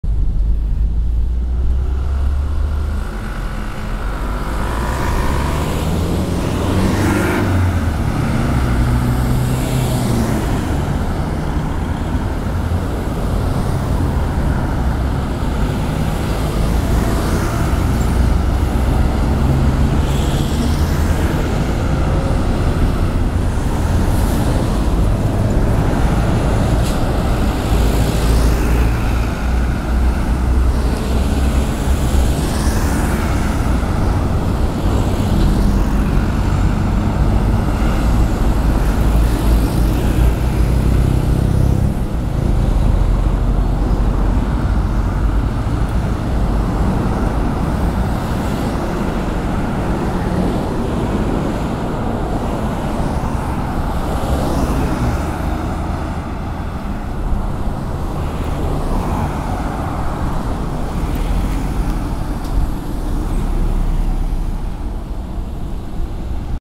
【4K24P・立体音響】交差点の音 #3 (2017.05.19)【SONY α6300 / Adphox BME-200】
4K Binaural Field Recording
4K映像・バイノーラルマイク・フィールド・レコーディング
立体音響 3D Sound （イヤホン推奨）

SONY α6300 (4K Video)
SONY SEL50F18 (Lens)
Adphox アドフォックス BME-200 (Binaural Mic)

マニュアルフォーカス